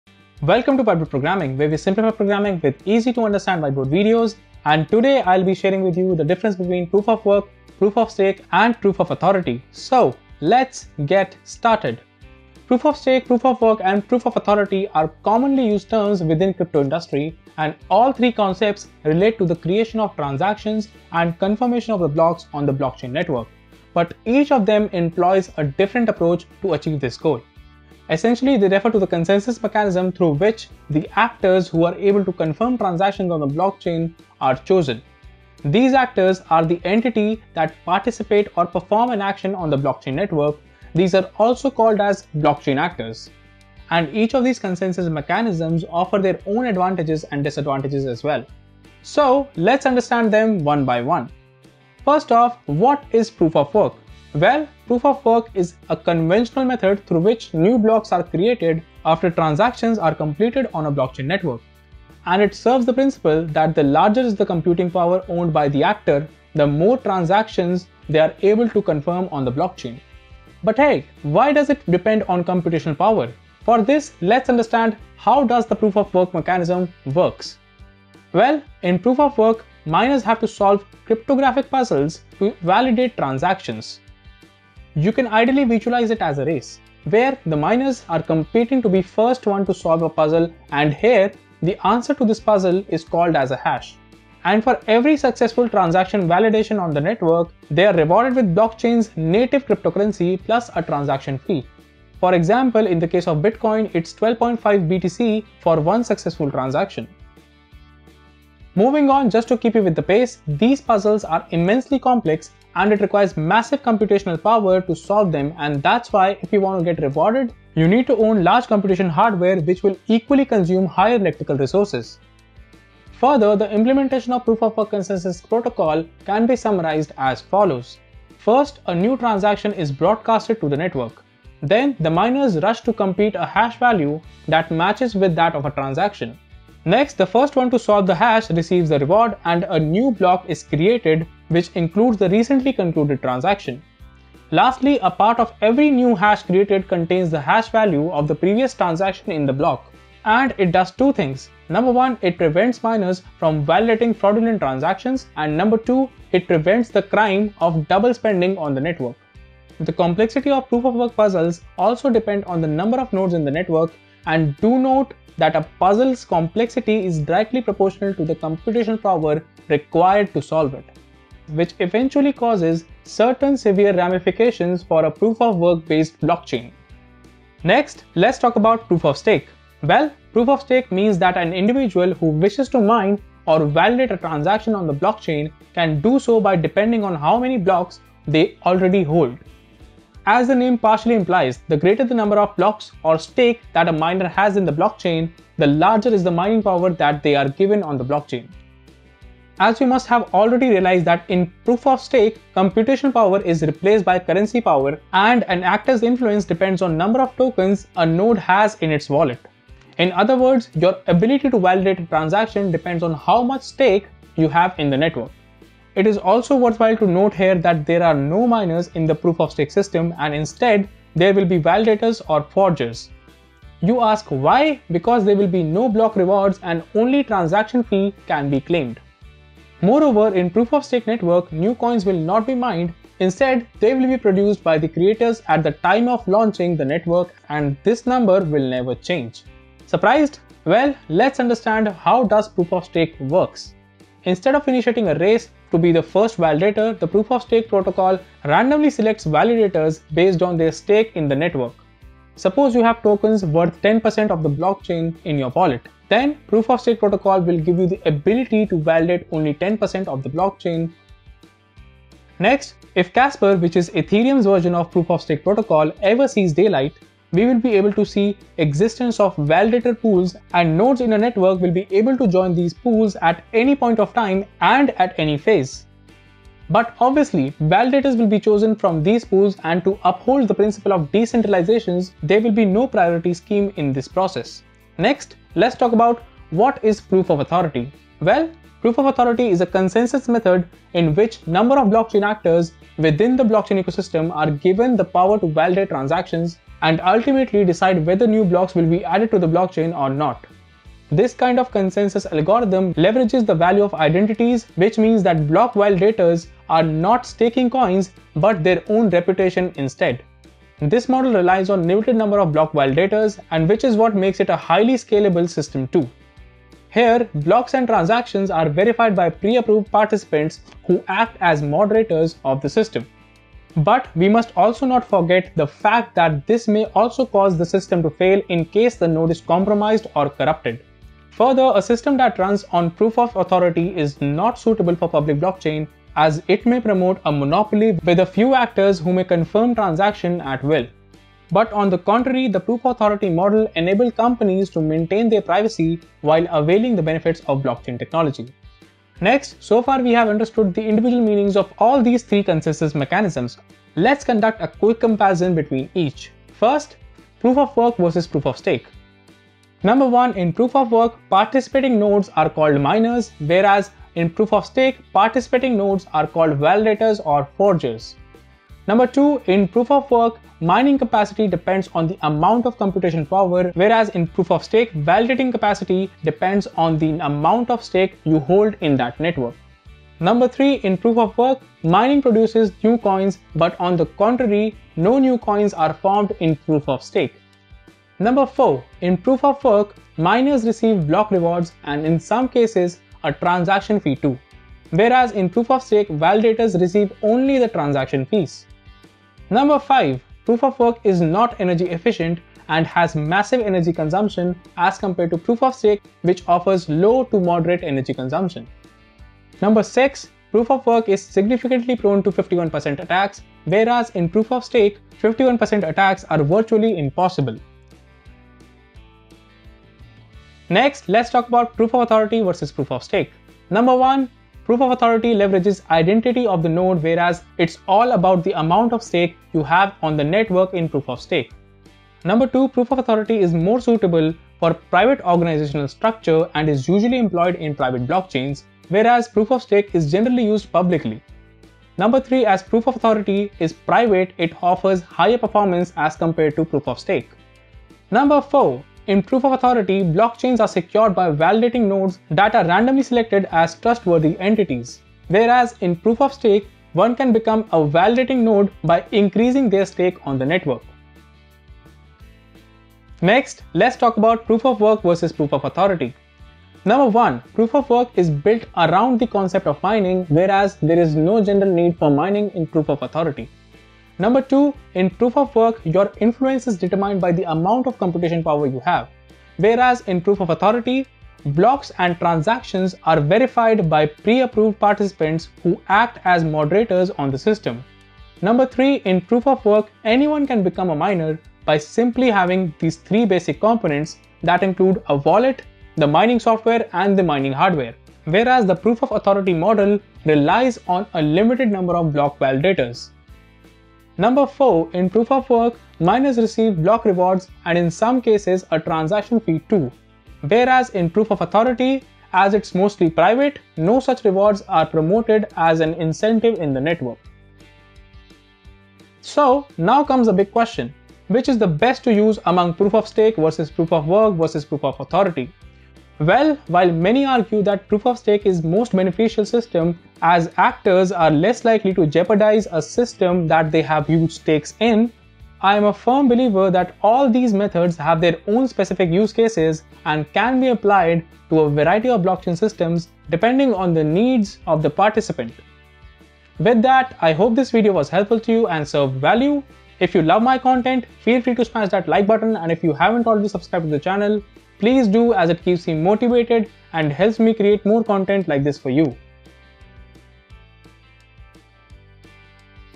Proof of Work vs Proof of Stake vs Proof of Authority Explained | PoA vs PoW vs PoS Blockchain
Get Certified in Blockchain Technology. Both tech and Non-Tech can apply!

What is Proof of Stake? What is Proof of Work? What is Proof of Authority? Do they even matter? Well, Proof of Stake, Proof of Work, and Proof of Authority are commonly used terms within the crypto industry. All three concepts relate to the creation of transactions and the confirmation of blocks on the blockchain but each employs a different approach to achieve this goal. This video will help you understand the difference between the three (PoS vs PoW vs PoA) along with what makes each of them unique.

Video Walkthrough (PoA vs PoS vs PoW Blockchain),
0:00 Introduction
0:24 What are Blockchain Actors & their Role in Consensus
0:52 What is Proof of Work Explained
1:15 How Does Proof of Work Mechanism Works
2:58 What is Proof of Stake Explained
4:01 How Does Proof of Stake Works
4:46 What is Proof of Authority Explained
5:56 Proof of Work vs Proof of Stake Difference (PoW vs PoS)
7:01 Proof of Authority vs Proof of Stake Difference (PoA vs PoS)
7:45 Proof of Work vs Proof of Authority Difference (PoW vs PoA)
8:40 The Best: Proof of Work vs Proof of Stake vs Proof of Authority?